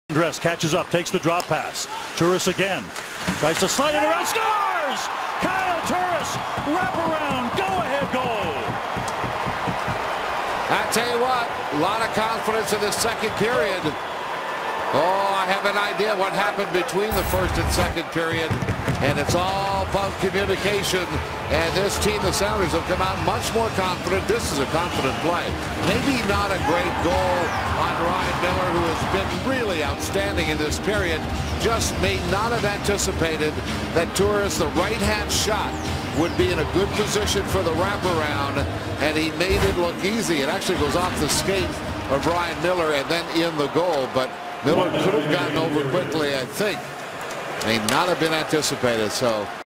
Kyle Turris Wrap-around Goal (Ottawa Senators vs Buffalo Sabres Mars 16, 2013) NHL HD
Kyle Turris wrap around goal, assisted by Latendresse.